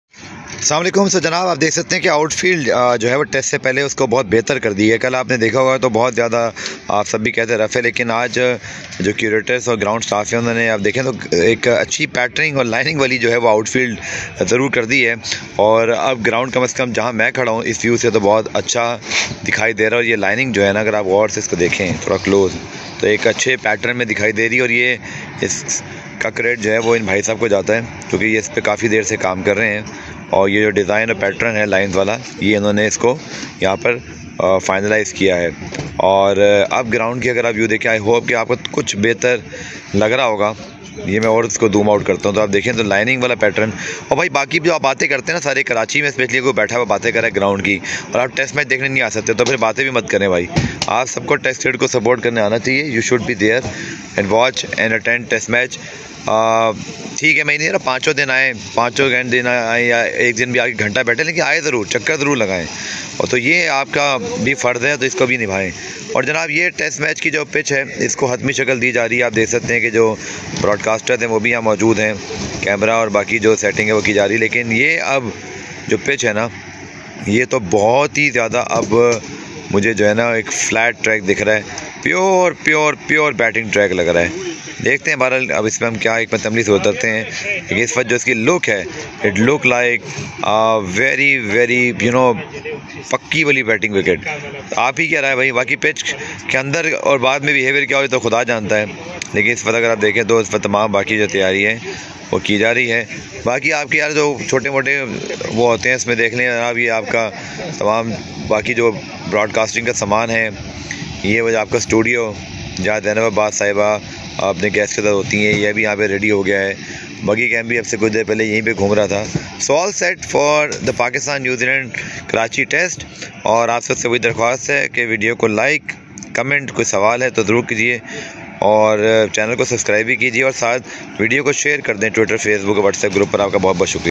New Beautiful look of National Stadium Karachi outffield ahead of Pakistan New Zealand 1st Test
Exclusive V Log from National Stadium Karachi on Pakistan vs New Zealand 1st Test
chief Selector Shahid Afridi visit National Stadium & Media Talk